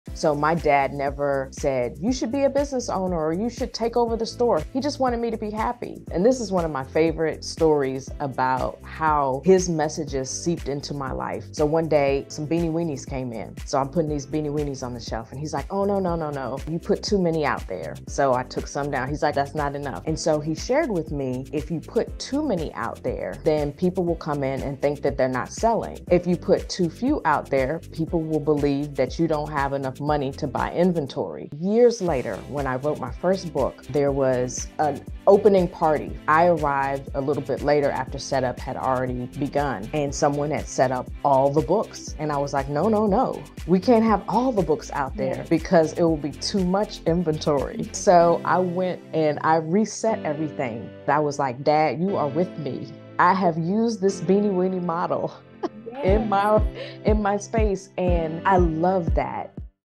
He told her THIS about the Beanie Weenees!
Natalie Hodge grew up surrounded by entrepreneurs, including her dad, who owned a small convenience store in Leatherwood, Virginia. His lessons on business help Nat to this day! nataliehodge.com